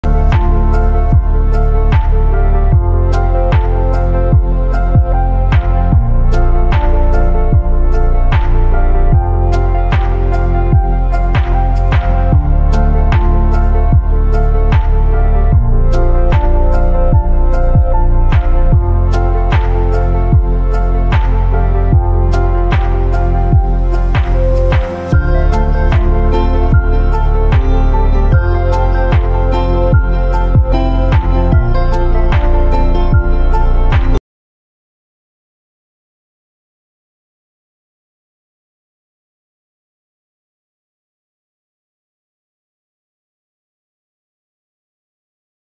KARIVADAM..Attapadi
Sillent valleyudae madithattiludae ozhuki attapadiyilethunna bavanipuzhayudae theeram...neerozhukku kuranjegilum puzhayudae   theerathinu saudharyam kuranjittilla...kallukalkond saudhariyameri karivadam bavanipuzhayudae oragal...